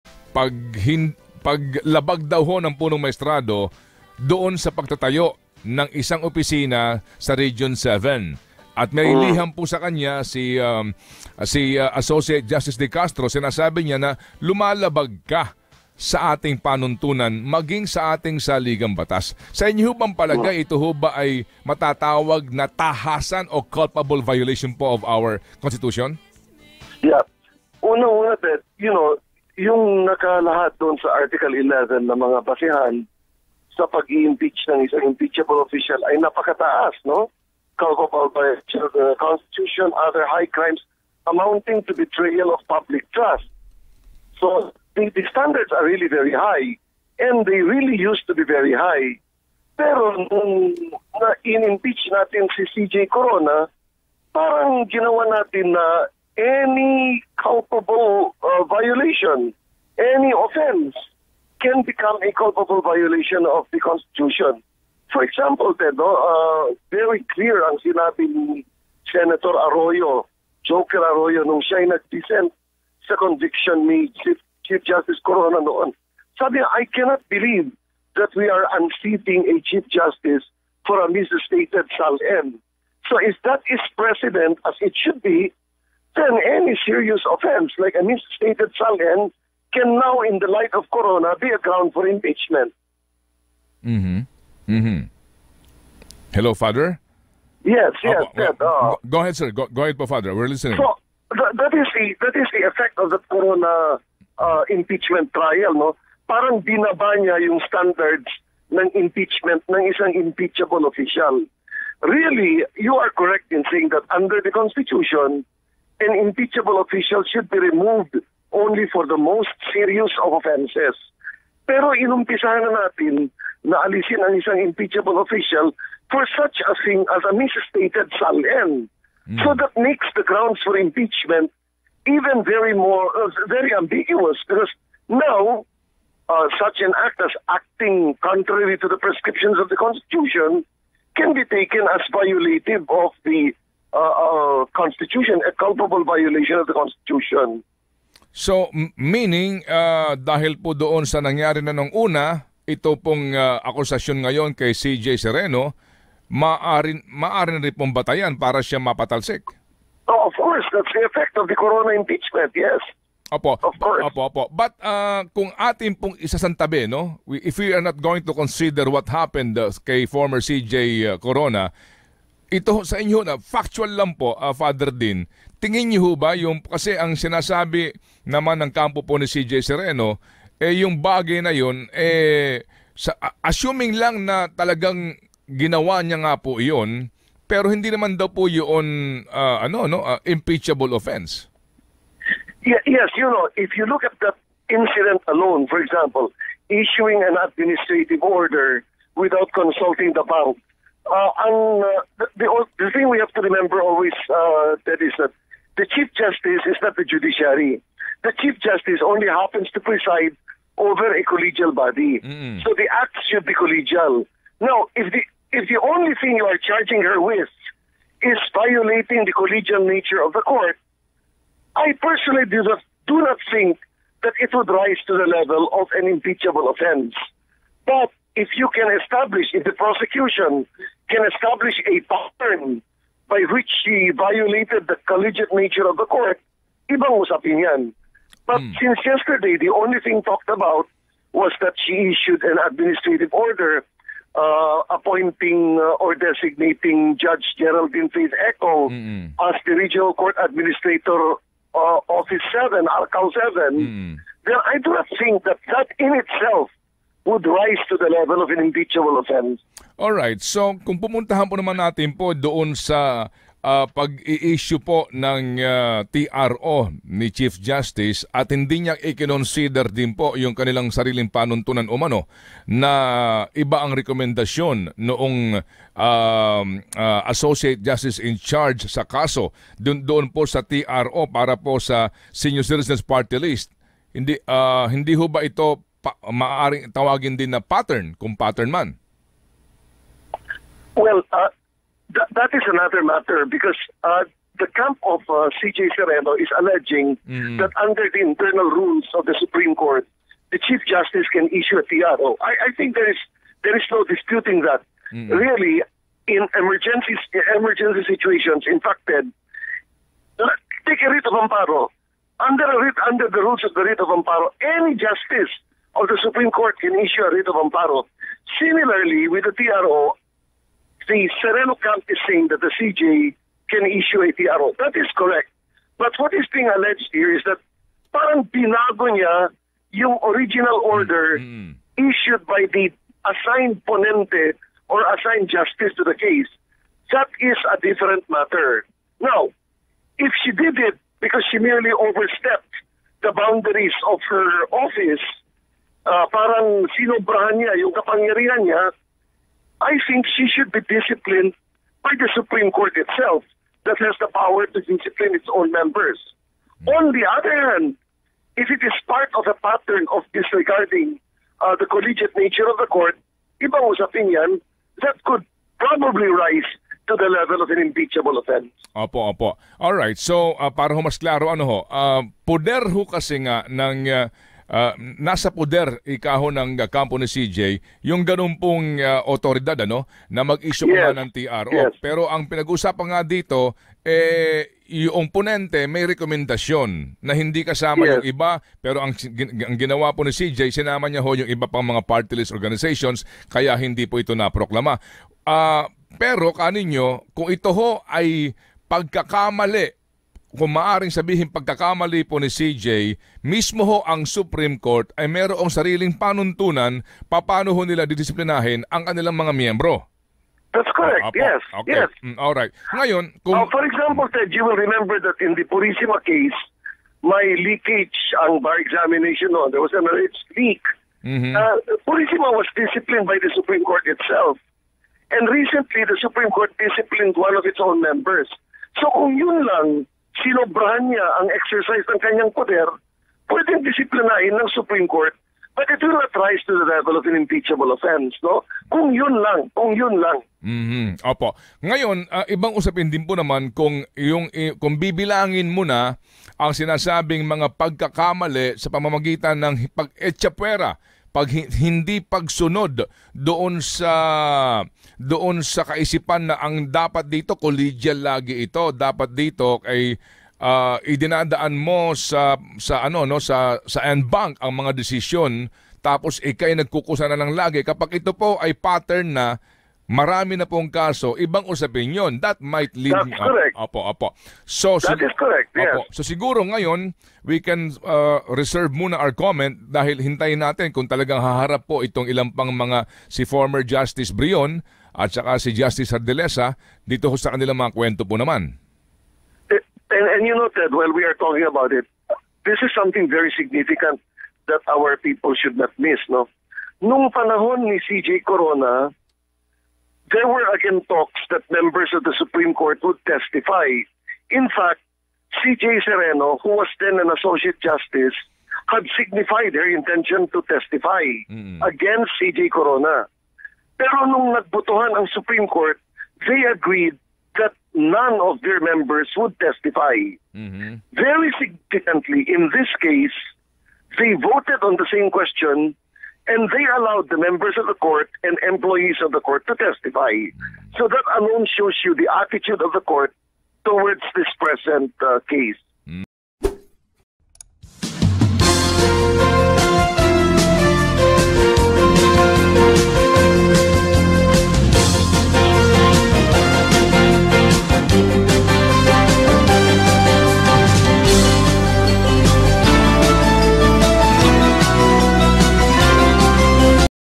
'Sereno's alleged unilateral actions impeachable if...'
MANILA - Allegations that Chief Justice Maria Lourdes Sereno had acted without the knowledge of the Supreme Court en banc can be an impeachable offense if it reveals a pattern of violating the tribunal's collegial nature, a law dean said Thursday.